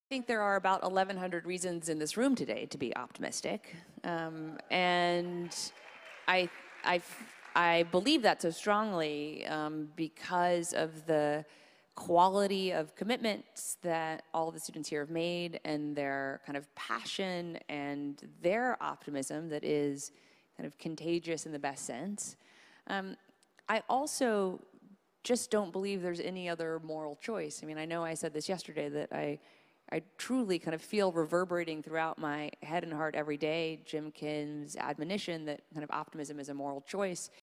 Chelsea Clinton on what keeps her inspired: "There are 1100 reasons in this room to be optimistic"
At the 2018 CGI University annual meeting, Chelsea Clinton spoke to over a thousand students about why they should be optimistic.